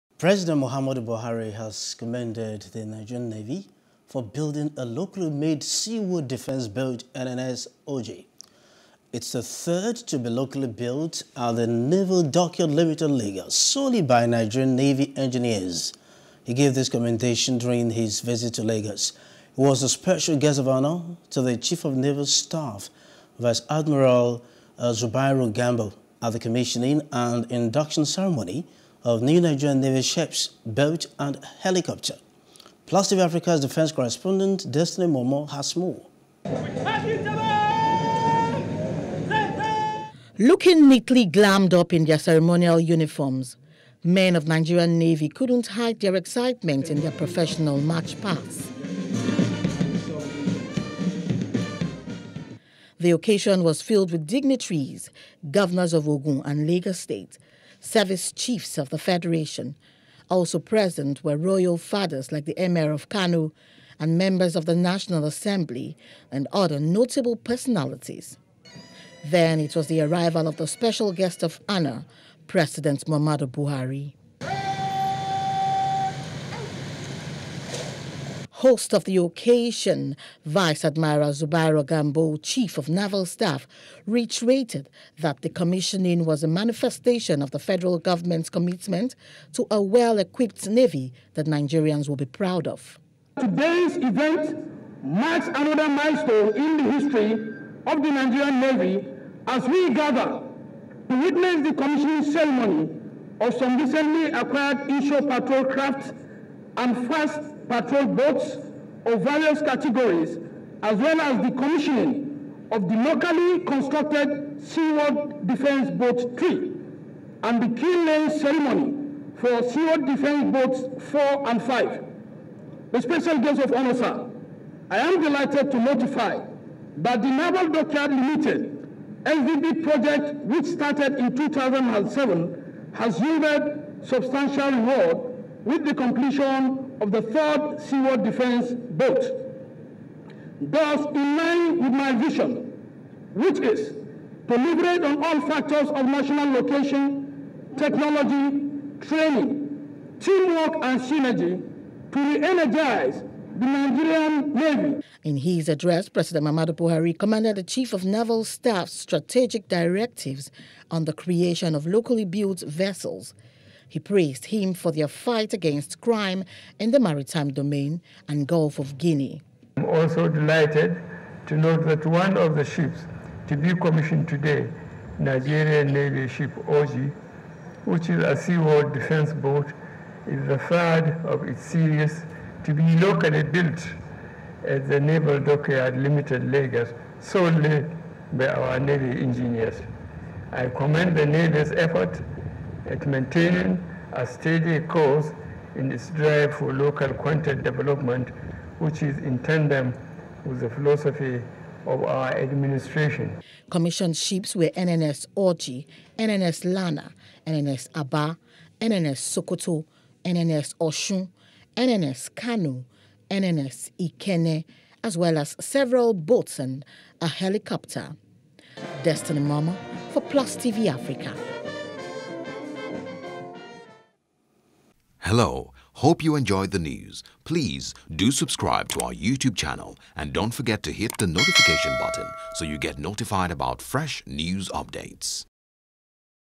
President Buhari Commissions Nigerian Navy Ships, Boat, Helicopter | NEWS
We are starting off on this report where president Muhammad Buhari visited lagos to commission some Nigerian Navy Ships, boats, and helicopter at the Naval Dockyard, Victoria Island